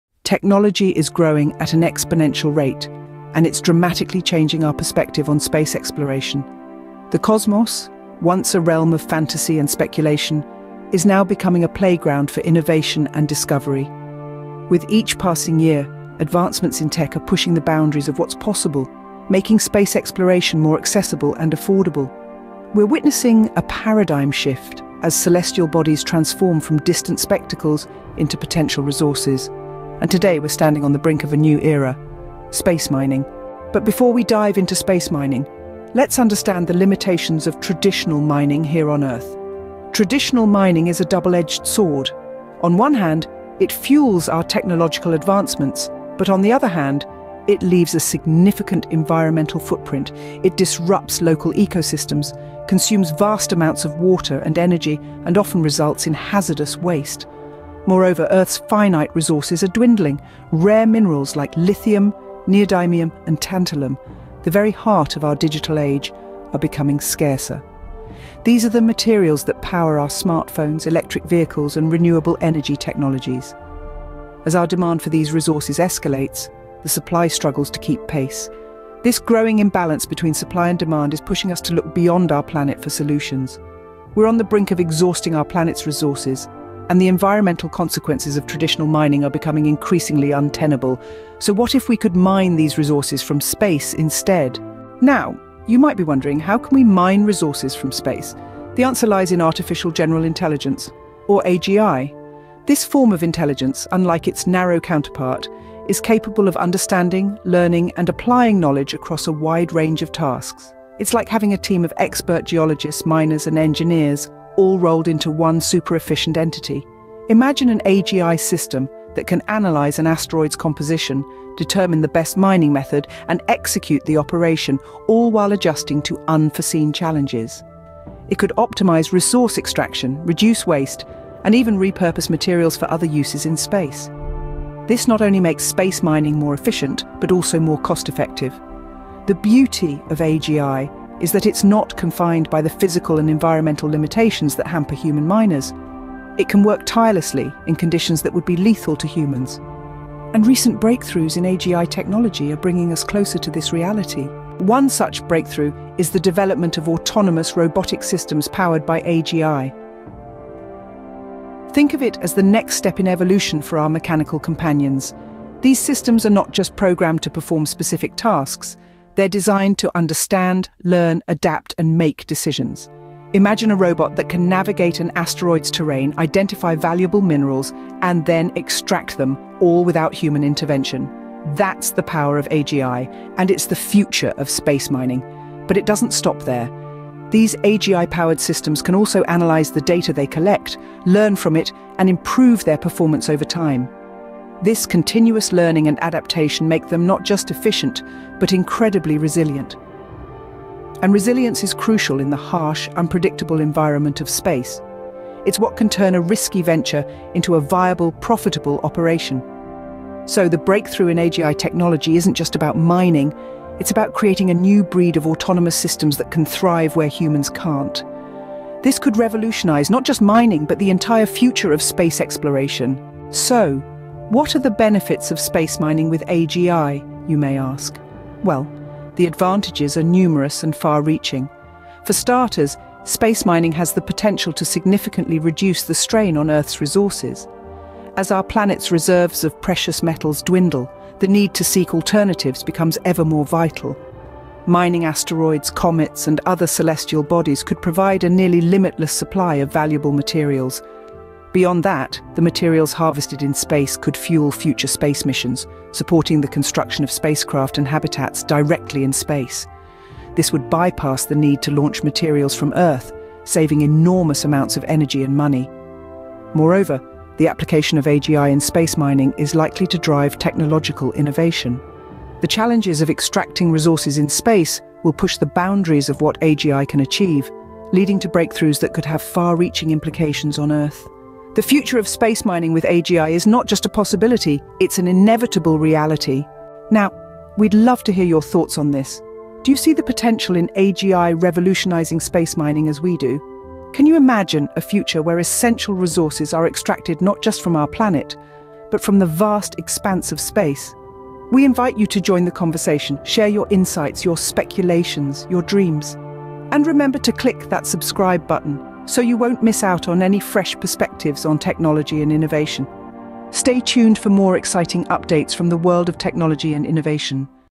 AGI: The Key to Future Space Mining
Discover the key to future space mining with AGI technology. Learn about the economic viability and transformative potential of resource extraction in the frontier of space exploration.
🚀 Welcome to the frontier of space exploration! In this captivating video, we delve into the revolutionary intersection of technology and cosmic ambition. Strap in as we explore the imminent rise of Space Mining powered by Artificial General Intelligence (AGI).

🛰️ From the moment humanity first gazed at the stars, we've dreamt of harnessing their boundless resources. Today, fueled by exponential technological growth, that dream is becoming a reality.

🌌 Our journey begins by illuminating the transformative potential of space mining. With Earth's resources depleting and demands skyrocketing, the cosmos beckons as our next frontier for rare minerals. But traditional mining methods fall short in the vastness of space, presenting a daunting challenge.

🌍 Enter AGI - the game-changer that could redefine our approach to resource extraction beyond Earth's confines. With its unparalleled cognitive abilities, AGI promises to revolutionize space mining, making it not just feasible but highly efficient and economically viable.

🤖 Join us as we unveil a pivotal breakthrough in AGI technology that promises to propel space mining into a new era. Witness the convergence of human ingenuity and machine intelligence as we stand on the brink of a cosmic leap forward.

🔑 This is the moment. This is the key to unlocking the untold riches of the cosmos.

🌟 In conclusion, we summarize the myriad advantages of employing AGI for space mining, underscoring its potential to reshape not only our world but the very stars themselves.

📣 Are you ready to be part of this cosmic revolution? The future is now. Let's reach for the stars, together! 🌠